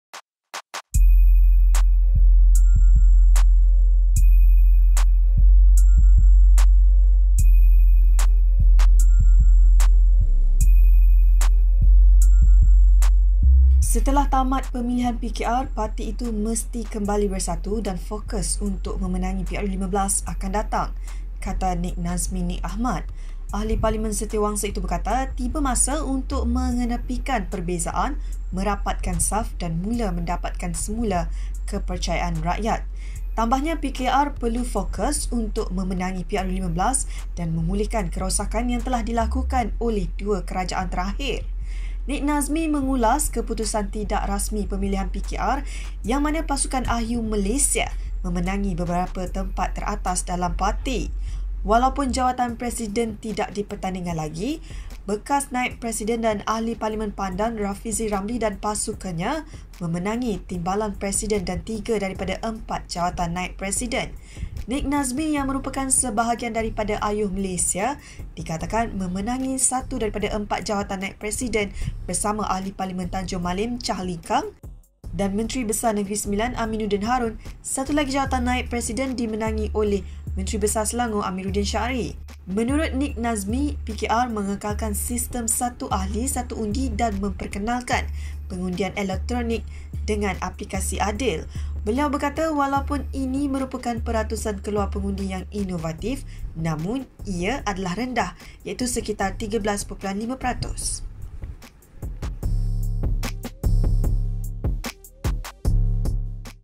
Rapatkan barisan, gandingan Anwar-Rafizi tiket terbaik menangi PRU15, kata Nik Nazmi
[BERITA PENUH]

Setelah tamat pemilihan PKR, parti itu mesti kembali bersatu dan fokus untuk memenangi PRU15 akan datang, kata Nik Nazmi Nik Ahmad.

Ahli Parlimen Setiawangsa itu berkata, tiba masa untuk mengetepikan perbezaan, merapatkan saf, dan mula mendapatkan semula kepercayaan rakyat.

Tambahnya, PKR perlu fokus untuk memenangi PRU15 dan memulihkan kerosakan yang telah dilakukan oleh dua kerajaan terakhir.